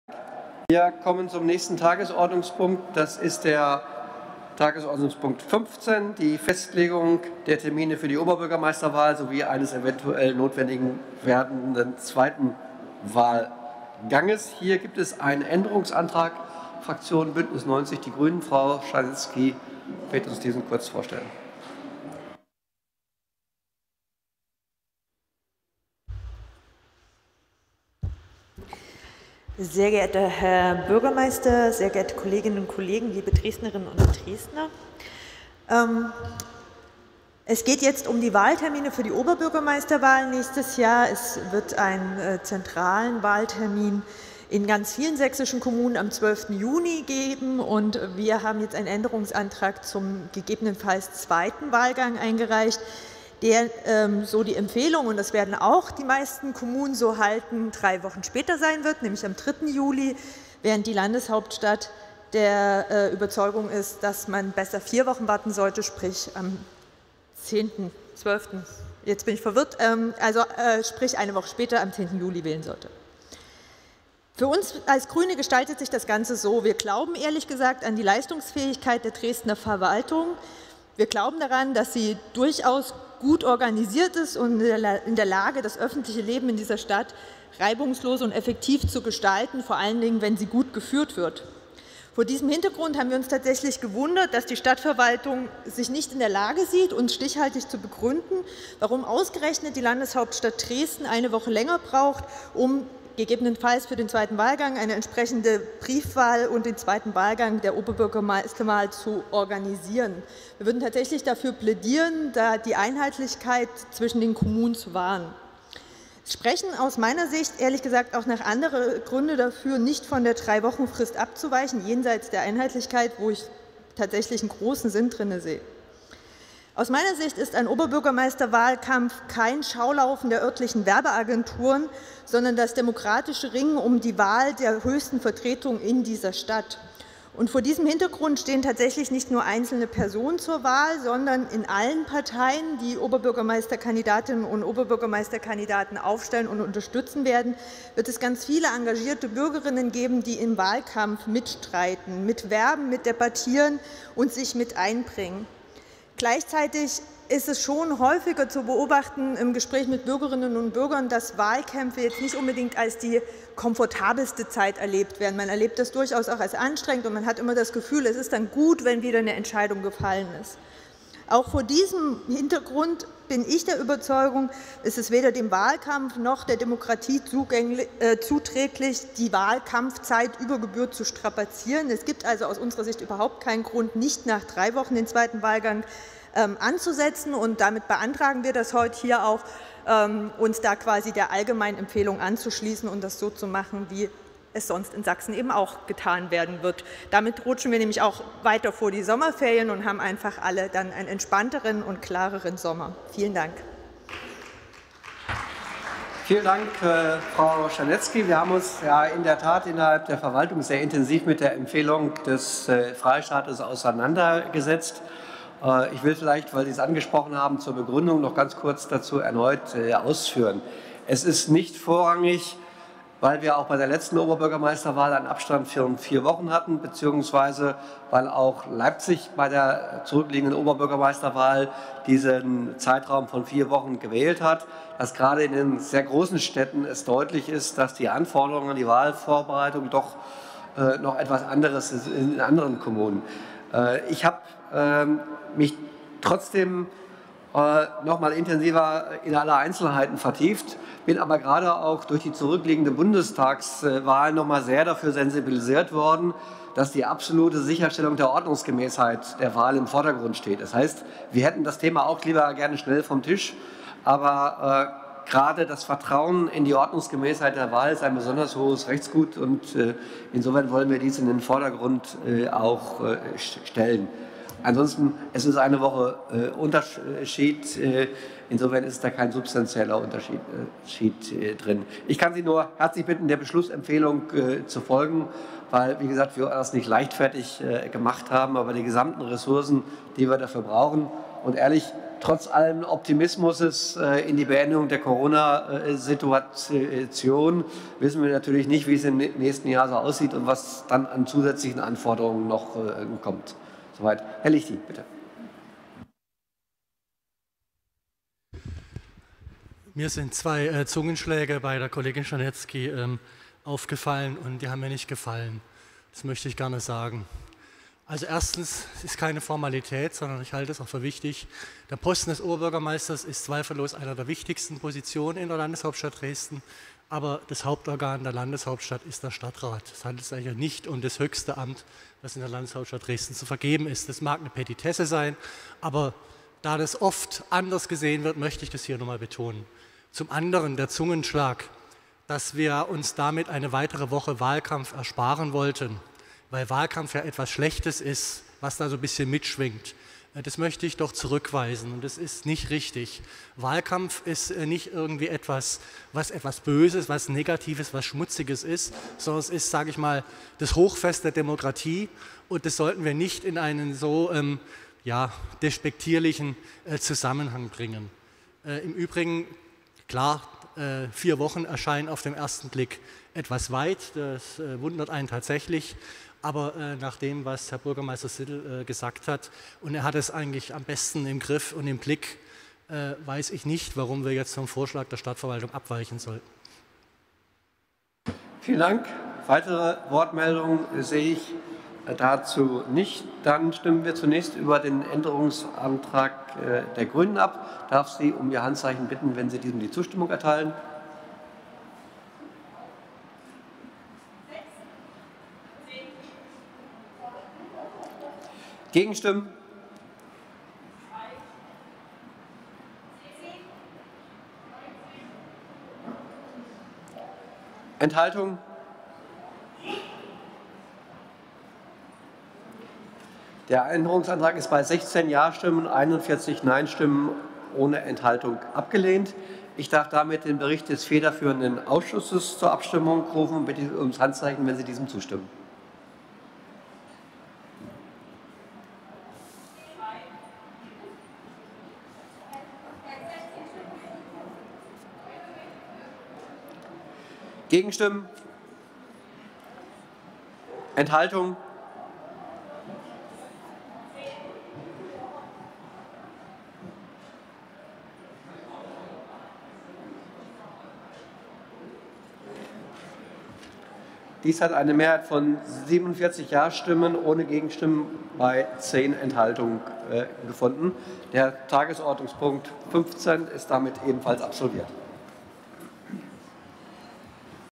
staDDrat am 16. Dezember 2021 - Festlegen der Termine für die Oberbürgermeisterwahl 2022
staDDrat Dresden Kommunalpolitik

Festlegen der Termine für die Oberbürgermeisterwahl sowie eines eventuell notwendig werdenden zweiten Wahlganges